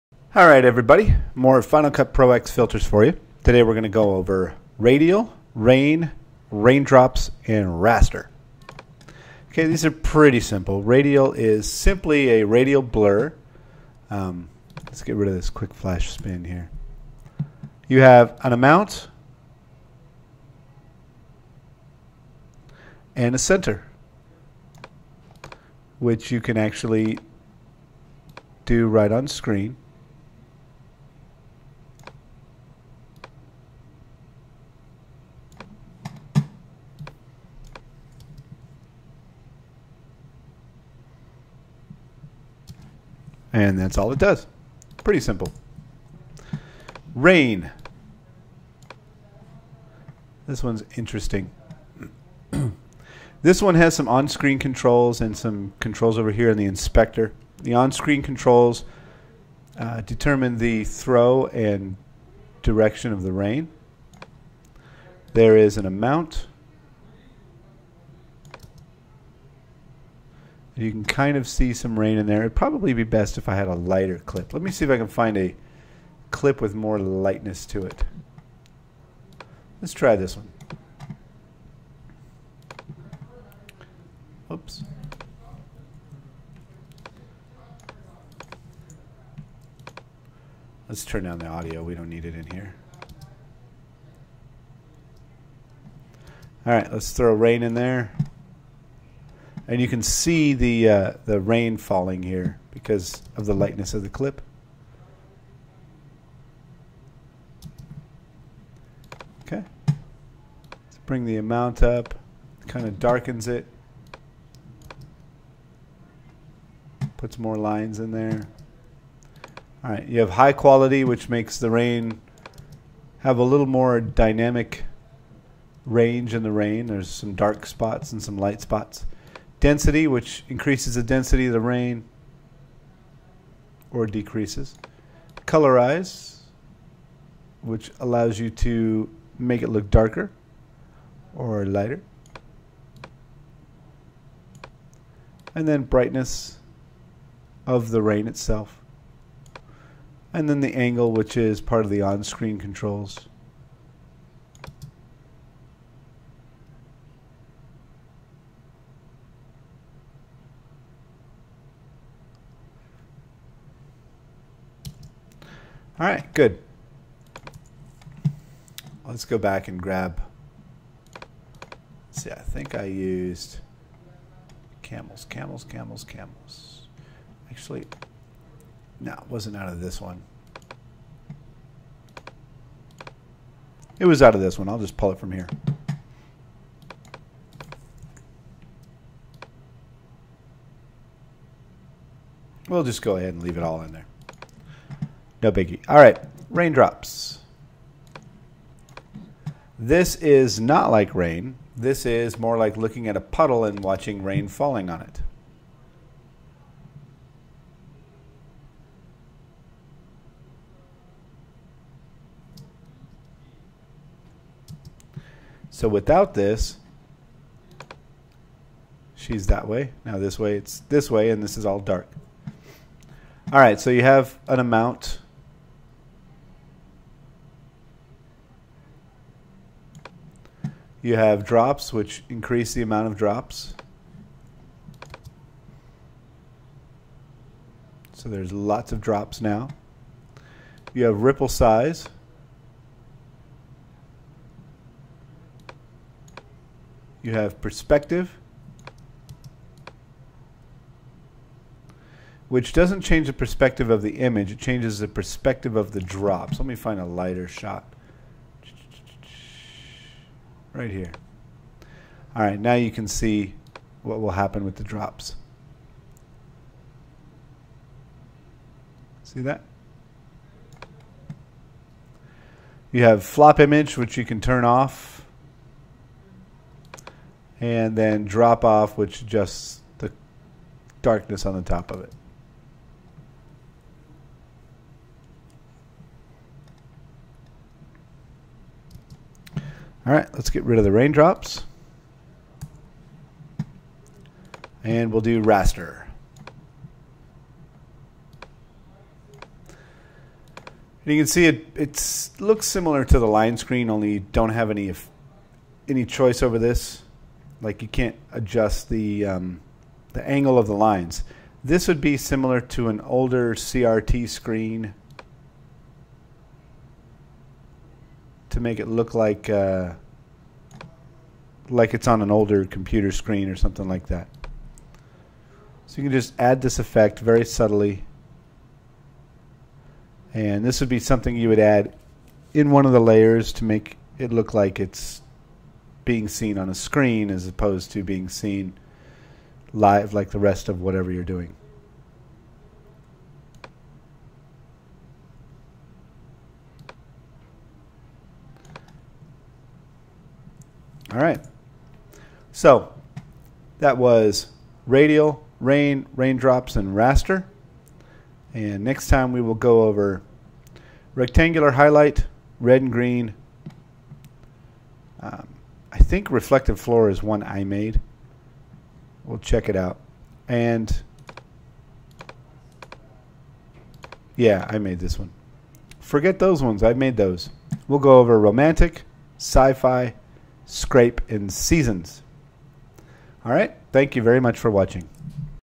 Final Cut Pro X Filters - Radial through Raster
Demonstrations of the following video filters in Final Cut Pro X:
Radial
Rain
Raindrops
Raster